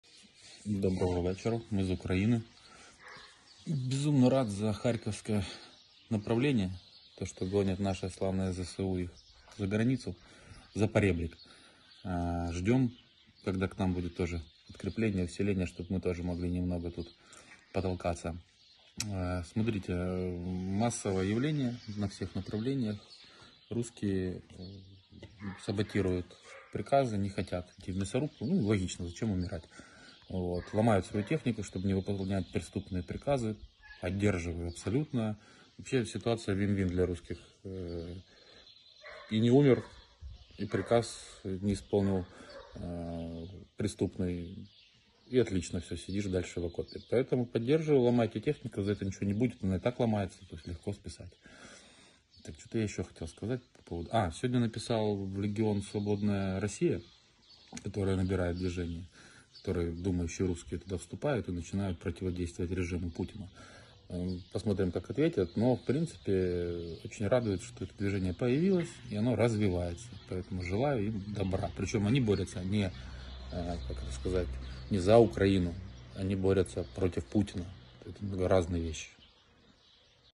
"росіяни саботують накази, не хочуть іти у мʼясорубку" - Віталій Кім (11.05.2022)
Начальник Миколаївської ОВА Віталій Кім про ситуацію в област